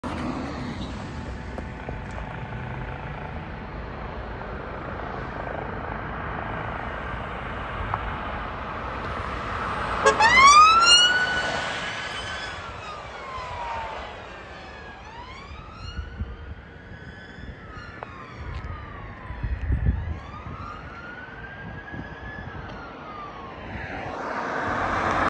North West Air Ambulance Rapid Response Vehicle Responding to a SERIOUS INCIDENT!
One of North West Air Ambulance’s HELIMED Cars seen here responding to an Unknown Incident!

The Air Ambulance is staffed by a team of Critical Care Paramedics & a Doctor who are very highly trained in the job they do and will respond to the most serious of incidents in the North West of England! Incidents include Stabbings, Traumatic Injuries, near amputations and serious road traffic collisions!

In this video:
HX001 | Air Ambulance Car
LB67 UNJ BMW X5